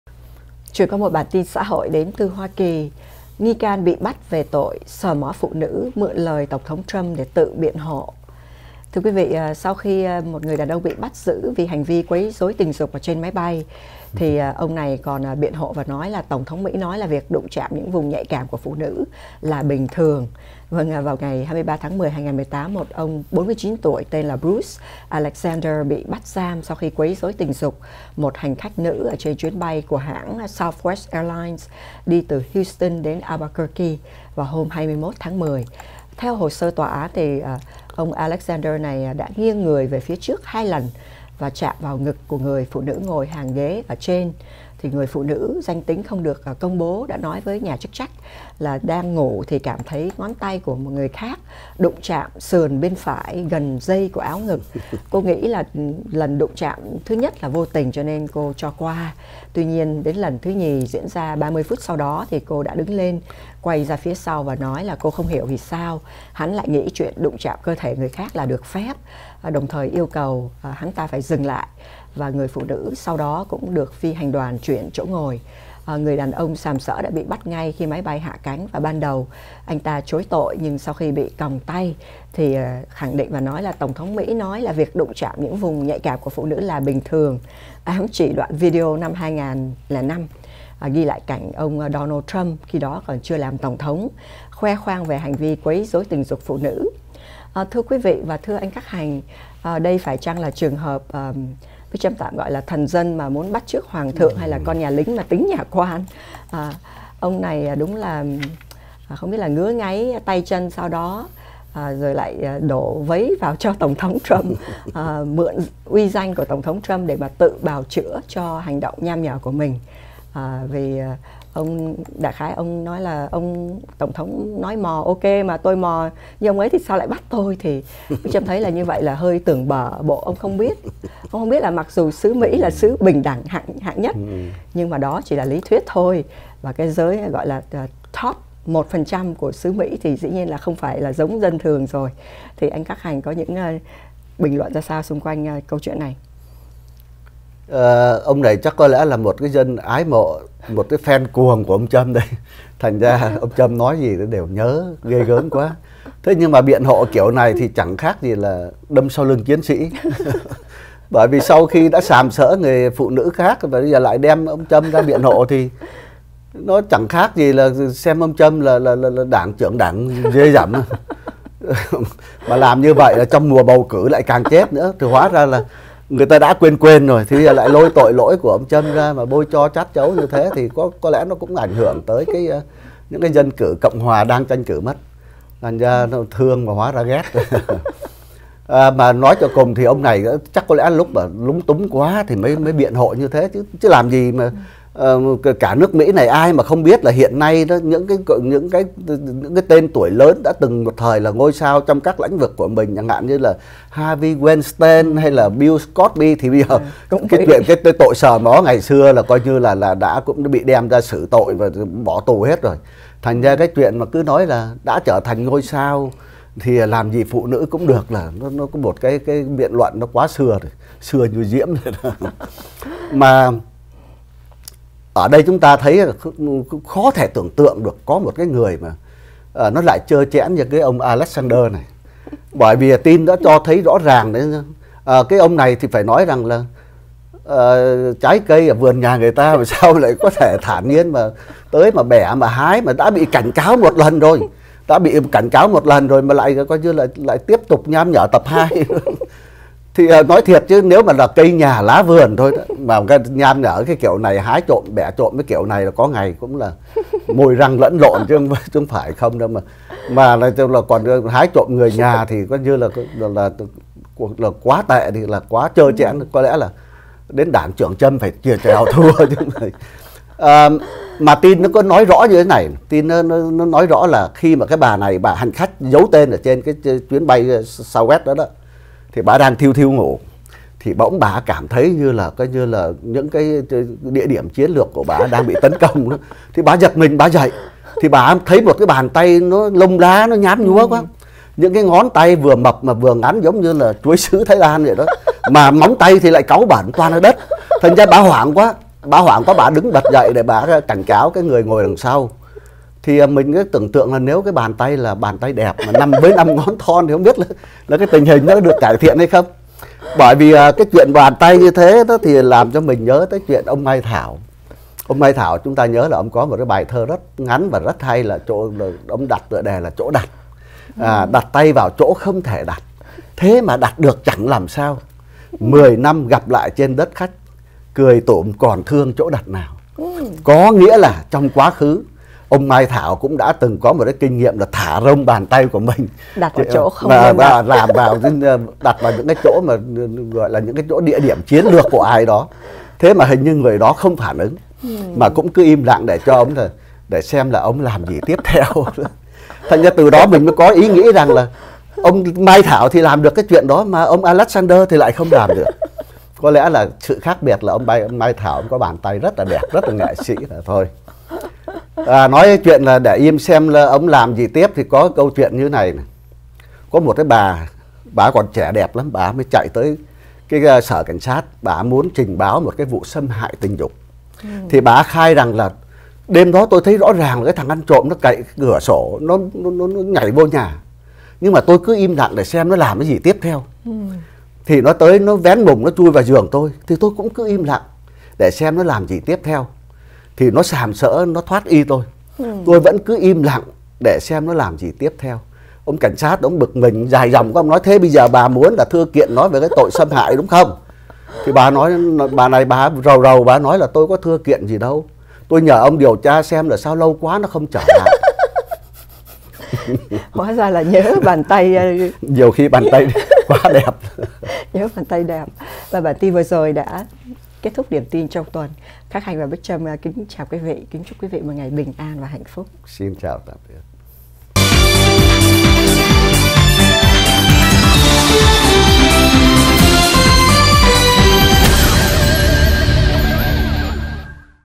📹 ĐIỂM TIN TRONG TUẦN: ▶ Nghi can bị bắt về tội sờ mó phụ nữ, mượn lời TT Trump tự biện hộ
SAIGON TELEVISION - Đài Truyền Hình Việt Ngữ Tại Hoa Kỳ